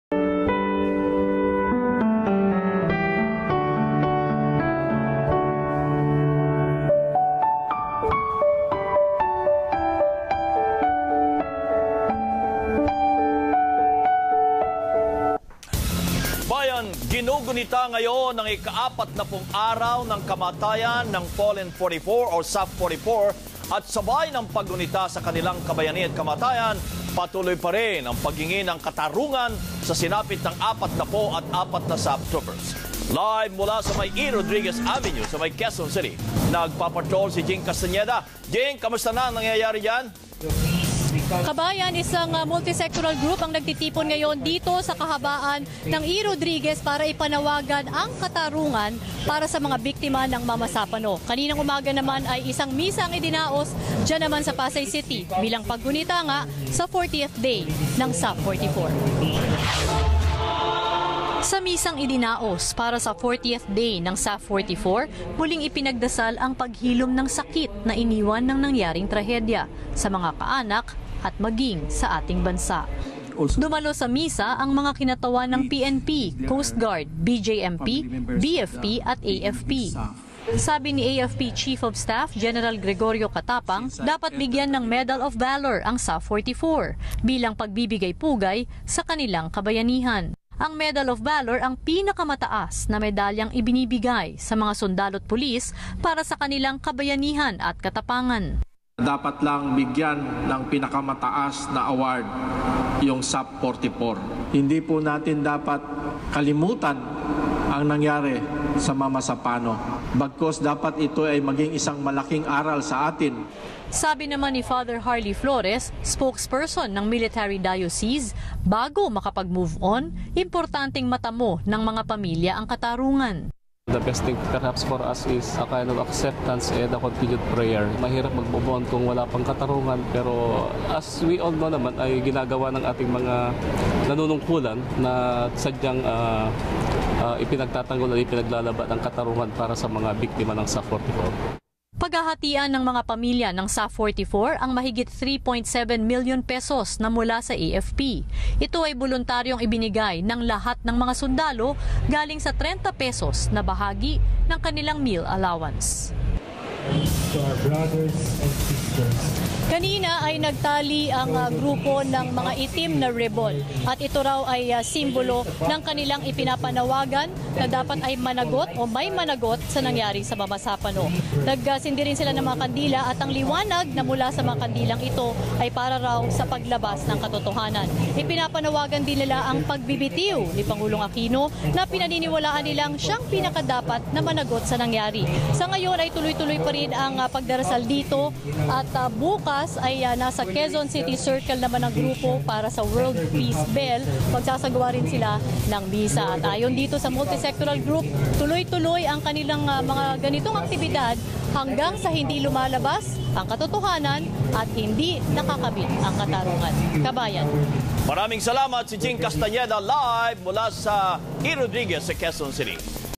Fallen Heroes Medal of Valor sa SAF 44, ginugunita
Ginunita ngayon ang ika-40 araw ng kamatayan ng Fallen 44. Sabay ng paggunita sa kanilang kabayanihan, patuloy pa rin ang paghingi ng hustisya sa sinapit ng 44 na Special Action Force (SAF) troopers. Iginiit ni Armed Forces of the Philippines )AFP) Chief of Staff Gen. Gregorio Catapang nararapat lamang na bigyan ng Medal of Valor ang SAF44 bilang pagbibigay-pugay sa kanilang kabayanihan.

Editor : ABS-CBN News Channel